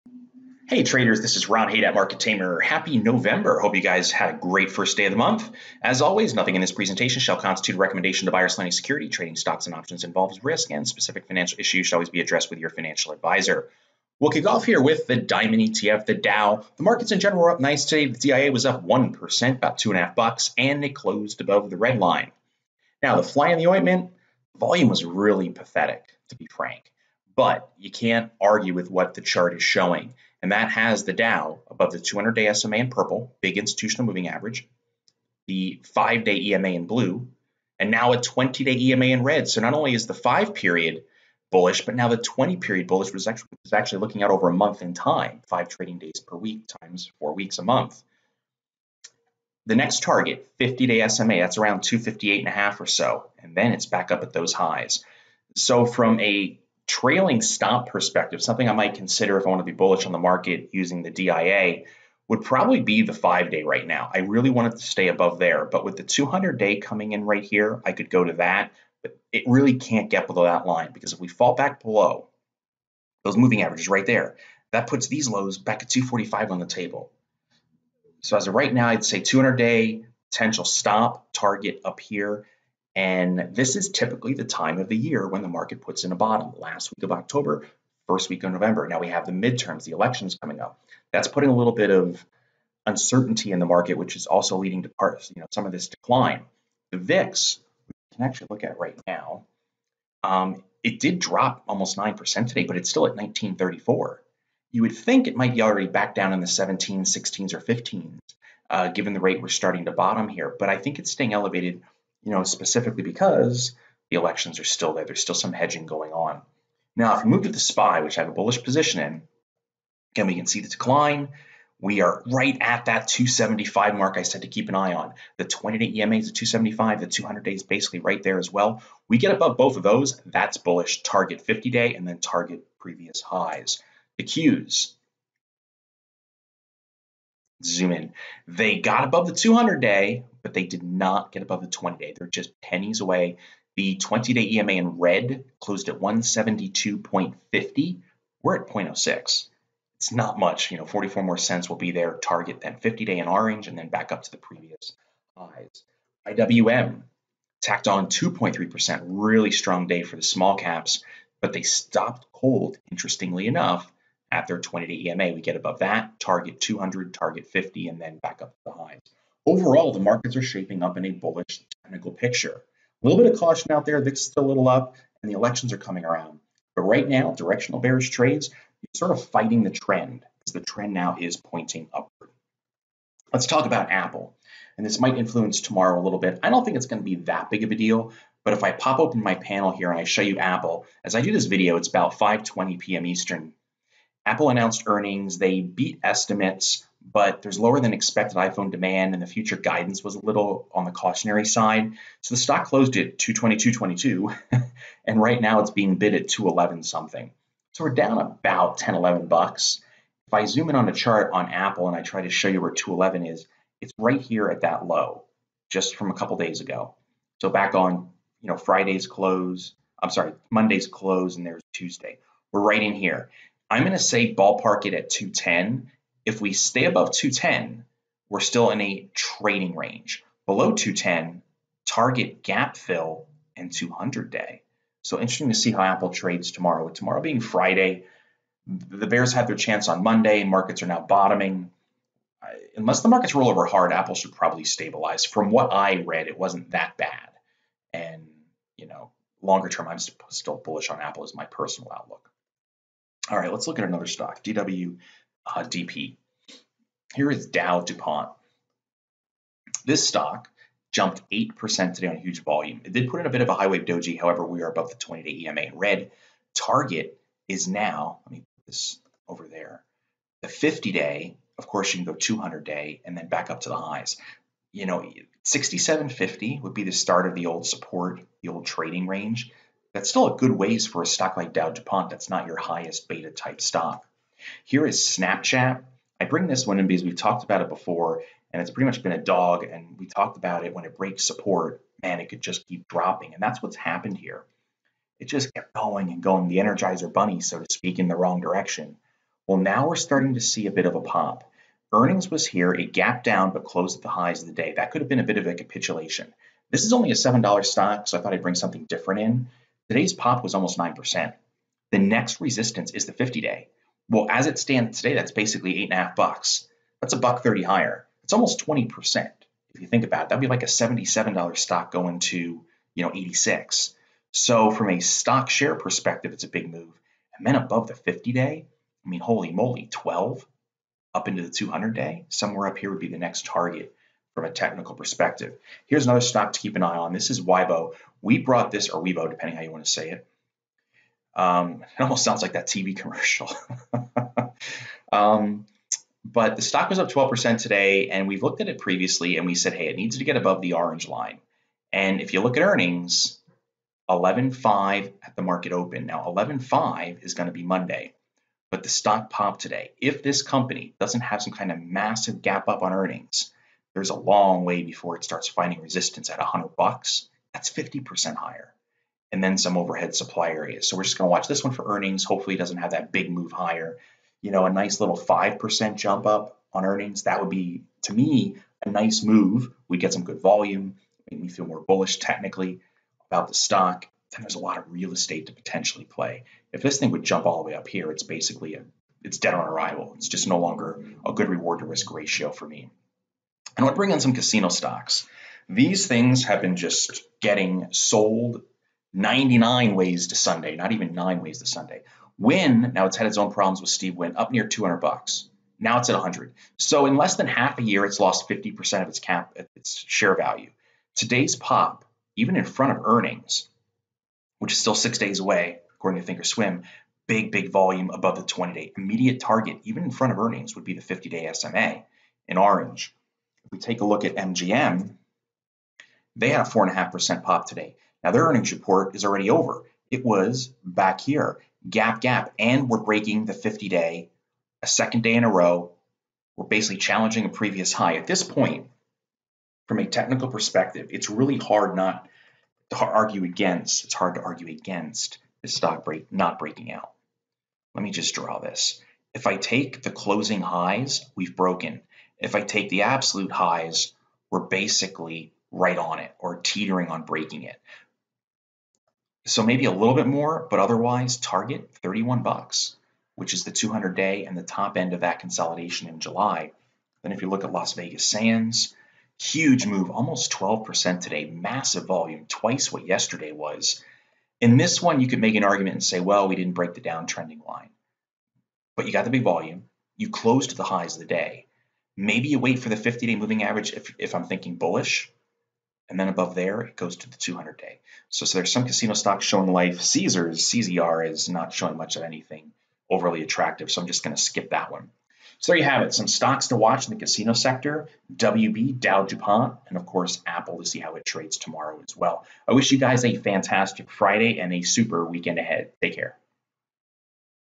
Nov 1 2018 - Apple Down on Earnings & Stocks To Watch
The major indexes finished higher again today, making it three days in a row.  The Dow ETF (DIA) even closed above its 20-day EMA.  The only fly in the ointment for the bulls was the lower volume.

Apple (AAPL) announced earnings after the bell and is currently trading down about $10.

We also highlighted some new 'stocks to watch' like Weibo (WB), DowDupont (DWDP) and even some casino stocks.

Have a fantastic Friday!